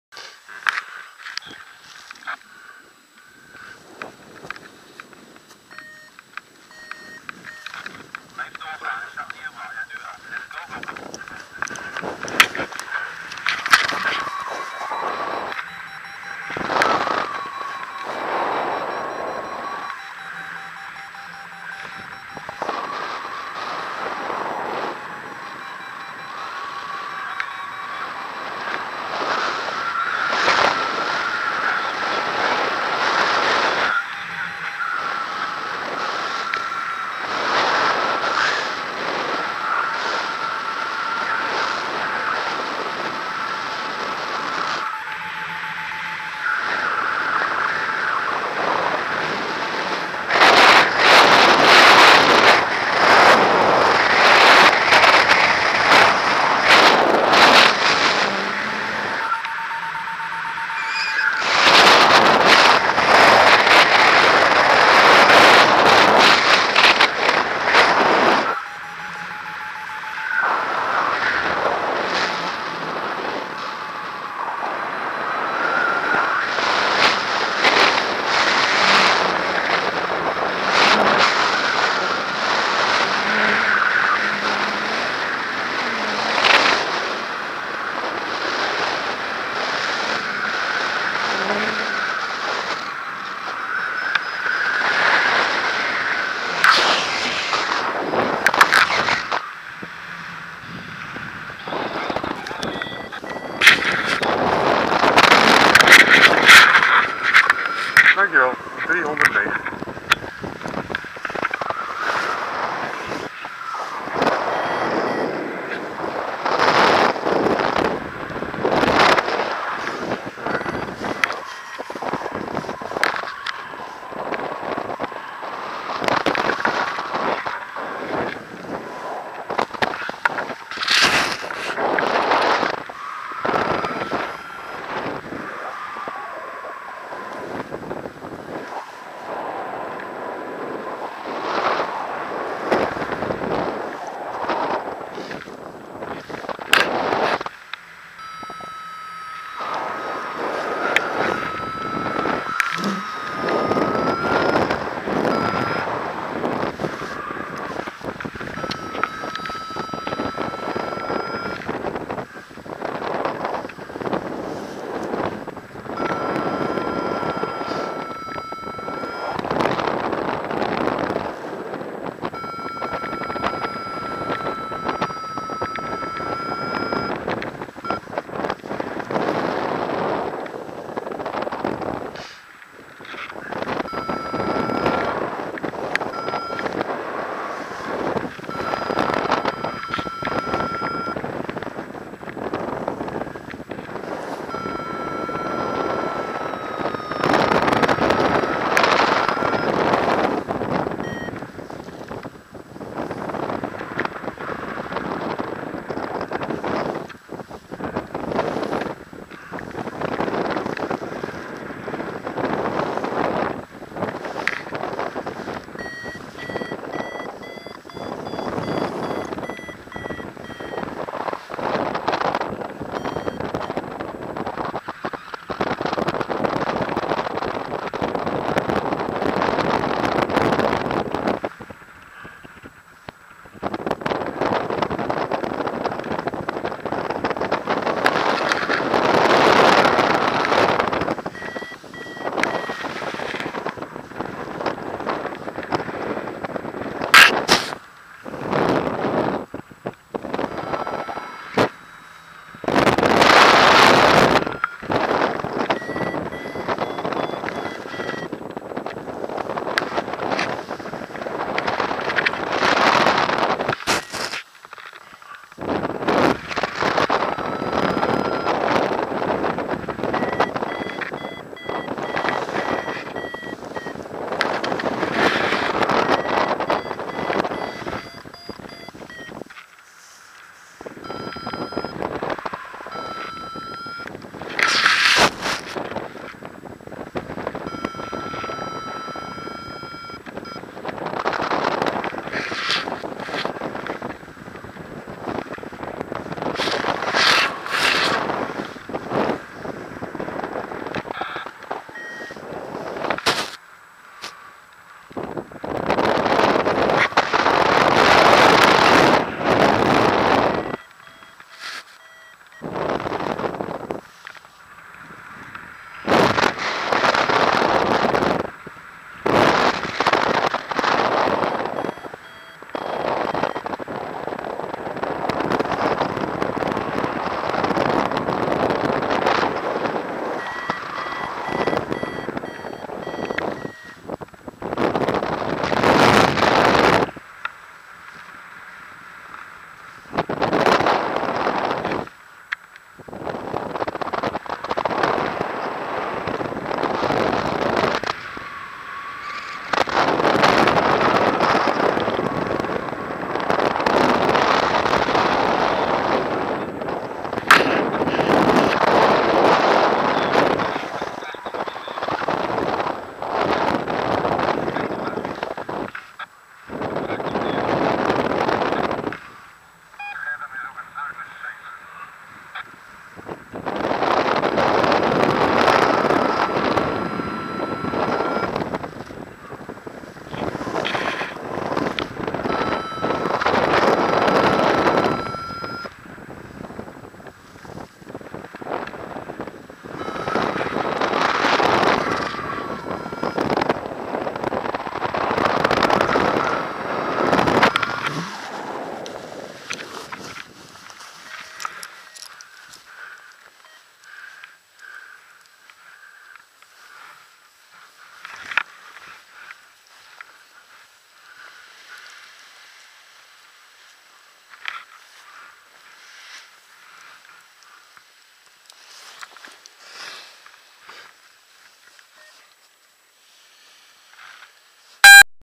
De eerste liervlucht 2015: Hooghalen 22 maart 2015
De eerste liervlucht van 2015. Het seizoen is geopend! :-)